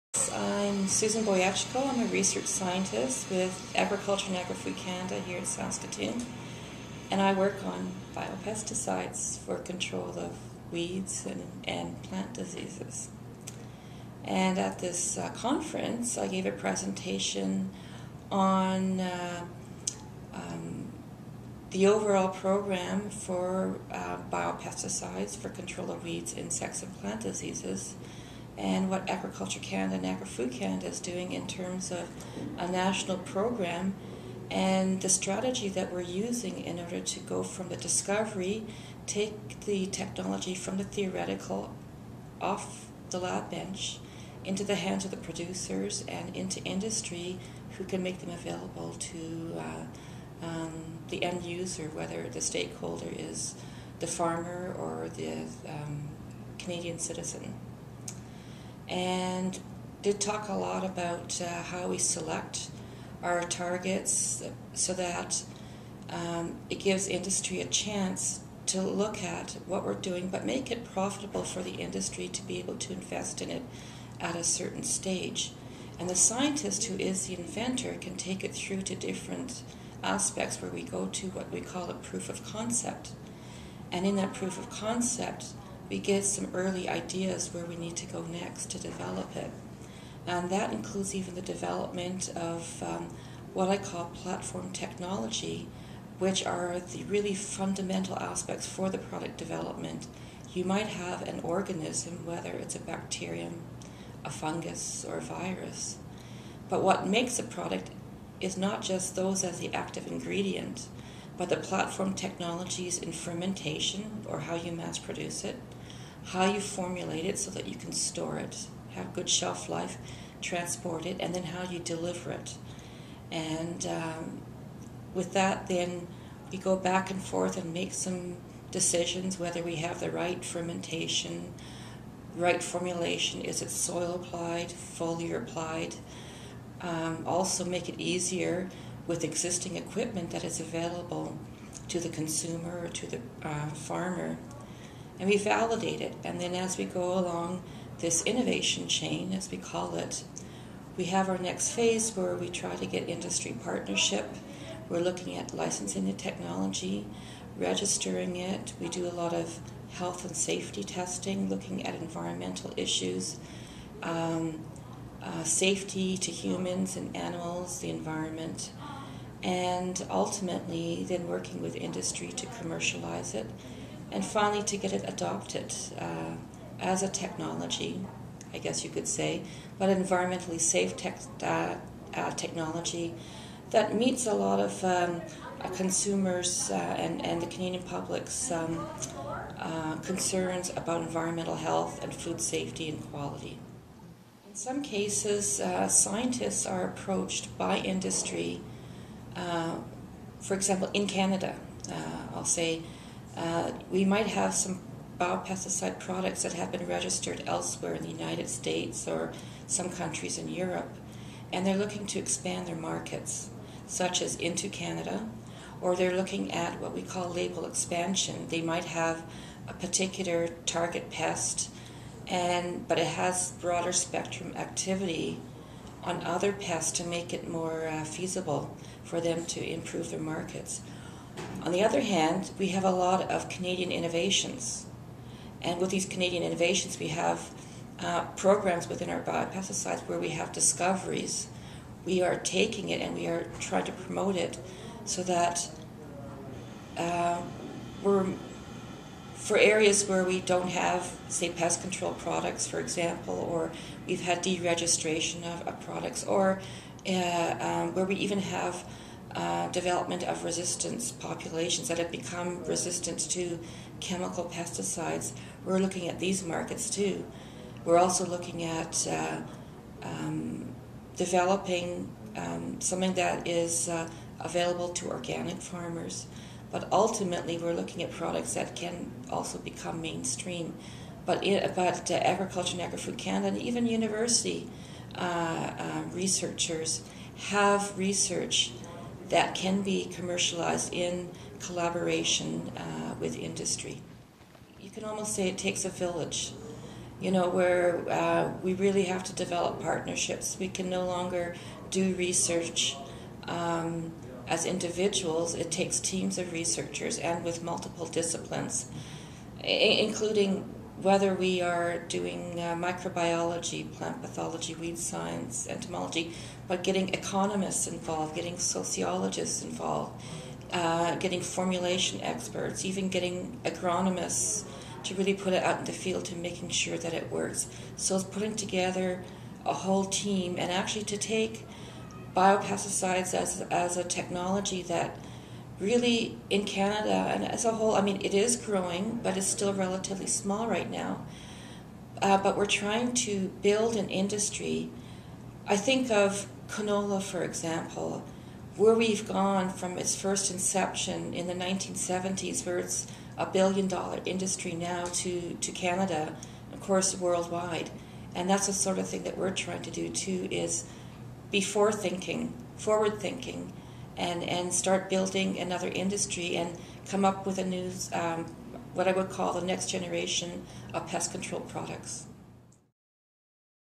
Sue Boyetchko at Biofumigation & Biopesticides Symposium hosted by Ag-West Bio in Saskatoon, SK
Susan Boyetchko discusses AAFC's program for development of bio-based pest controls for agriculture